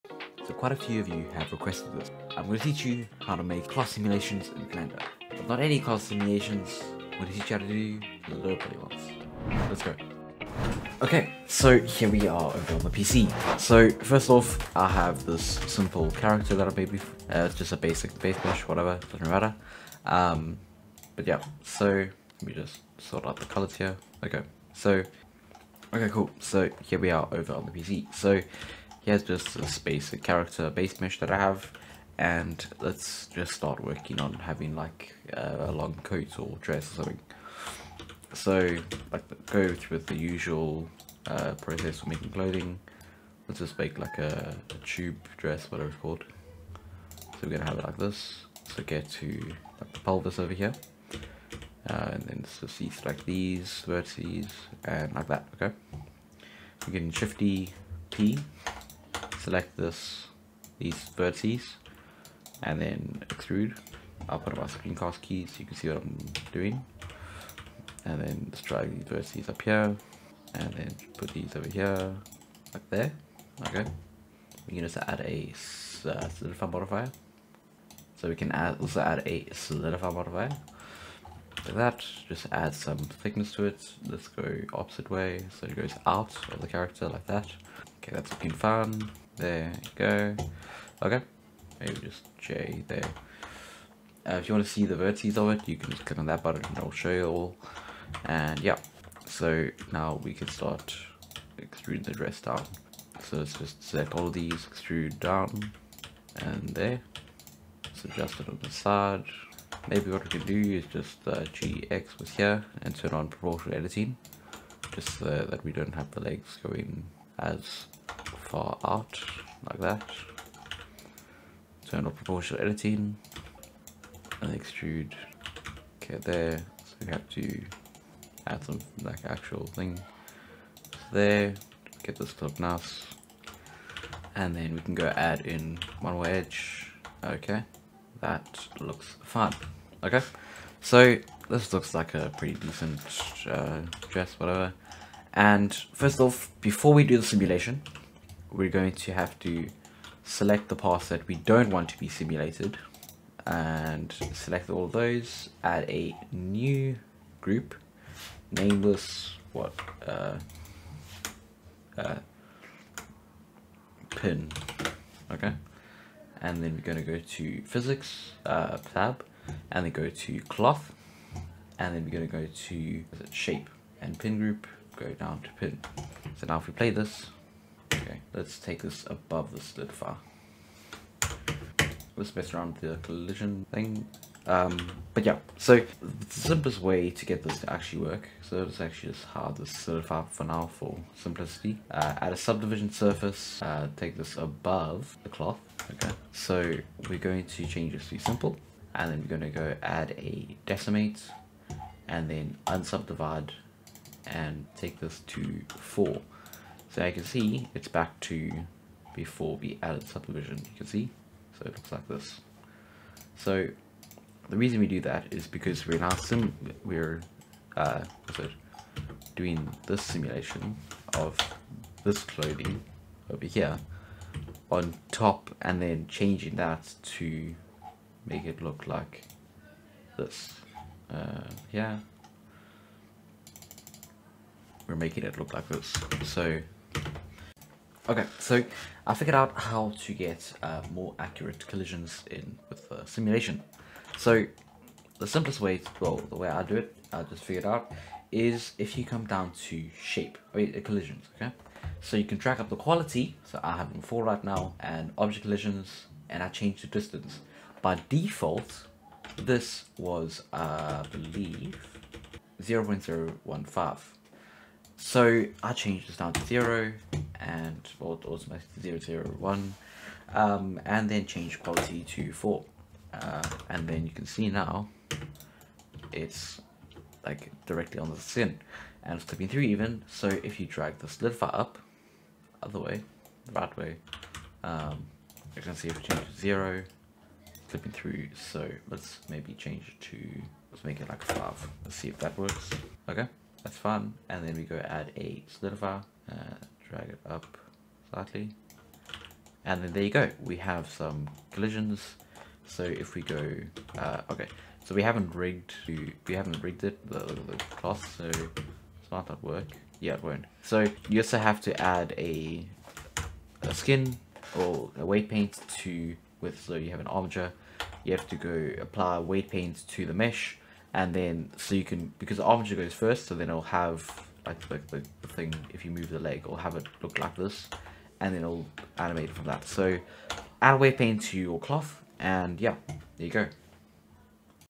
How to make LOW POLY cloth sims - Blender guide.
How to make low poly simulations for low poly clothing and other cloth type meshes.

TimeStamps:

0:00 - Intro
0:10 - Making some clothing
2:27 - How to make a simulation
3:57 - Fix mesh collisions

The style of this video is similar to Grant abbit and Imphenzia in parts. A large influence on this series has come from content creators like CG cookie and CG geek